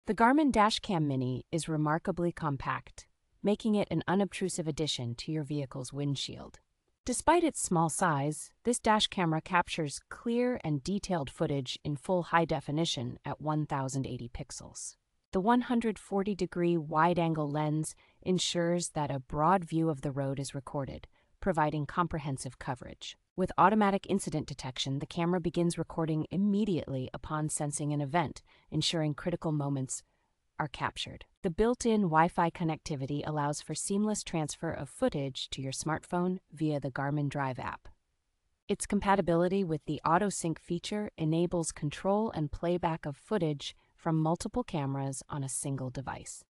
REVIEW (2024): Garmin Dash Cam Mini 010-02062-00. ESSENTIAL details.
The Garmin Dash Cam Mini is remarkably compact, making it an unobtrusive addition to your vehicle's windshield.

Despite its small size, this dash camera captures clear and detailed footage in full high definition at one thousand eighty pixels.

The one hundred forty-degree wide-angle lens ensures that a broad view of the road is recorded, providing comprehensive coverage.

With automatic incident detection, the camera begins recording immediately upon sensing an event, ensuring critical moments are captured.

The built-in Wi-Fi connectivity allows for seamless transfer of footage to your smartphone via the Garmin Drive app.

Its compatibility with the auto sync feature enables control and playback of footage from multiple cameras on a single device.